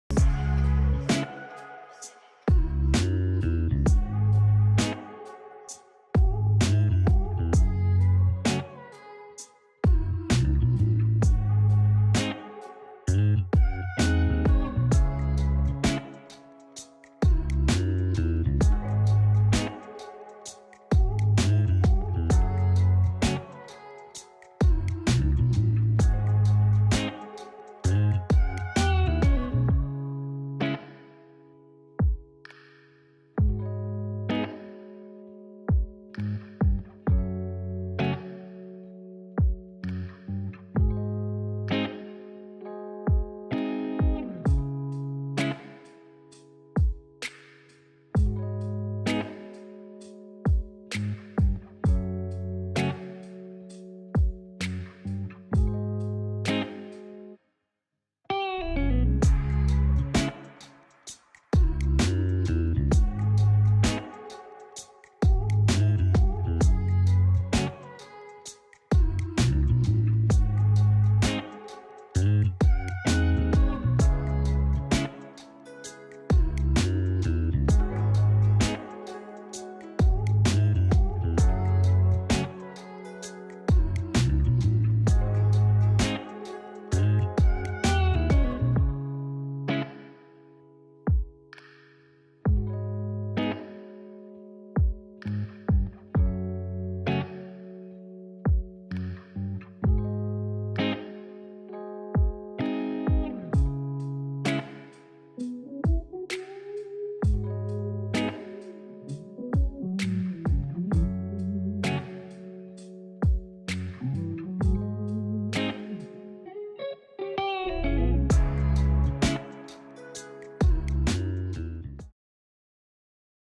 House Tour | Dúplex en Neguri, Getxo | Casas en venta en Bizkaia | Inmobiliaria de Lujo
¡Ven y descubre este espectacular dúplex a la venta en una prestigiosa urbanización del centro de Neguri, Getxo.